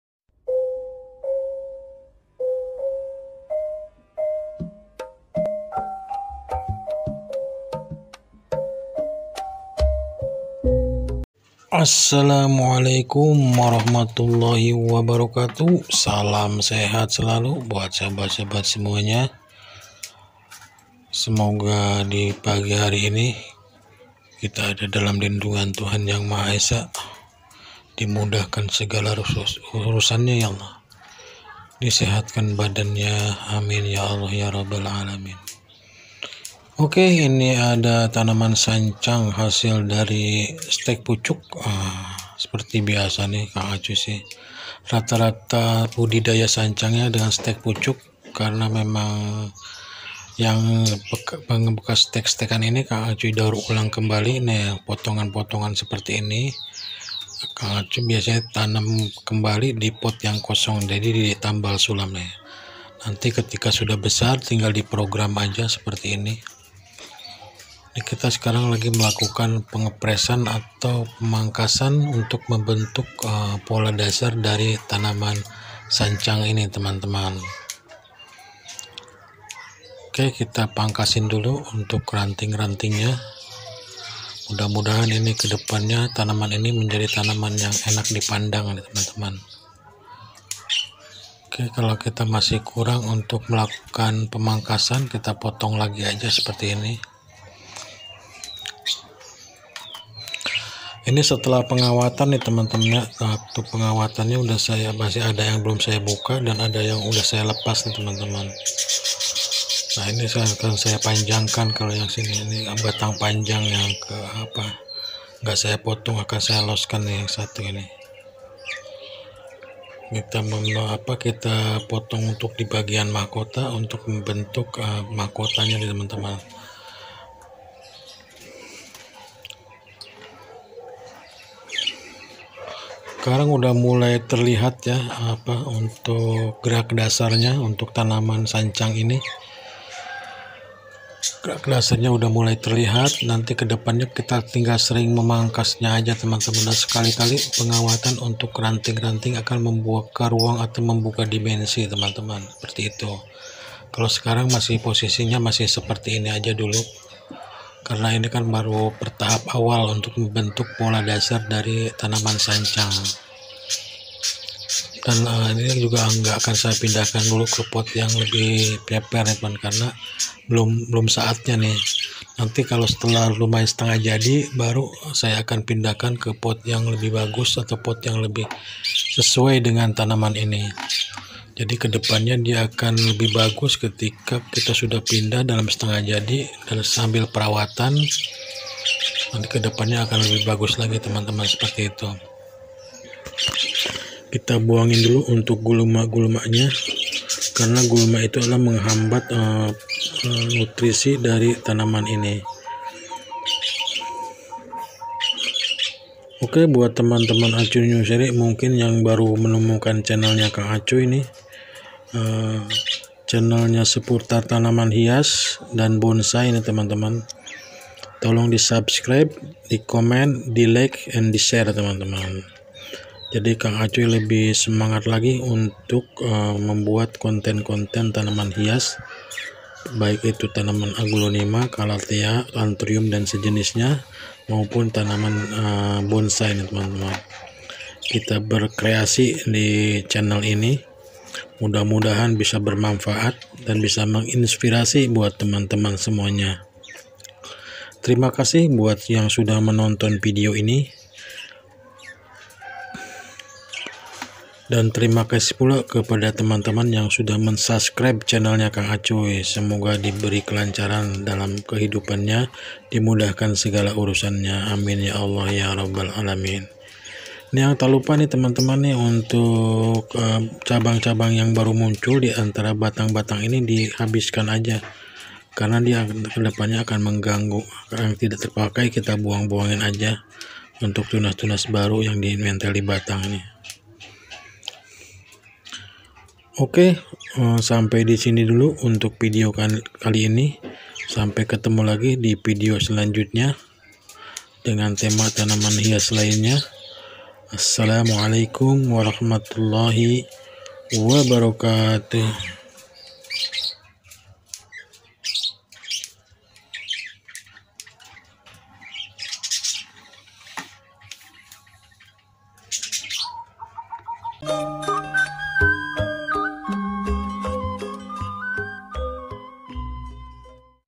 saatnya pengepresan sancang
saatnya pengepresan tanaman sancang,untuk membentuk pola dasar bonsai sancang.
tanaman ini hasil stek pucuk cukup lumayan lama untuk memprogramnya harus sabar menunggu pembesaran.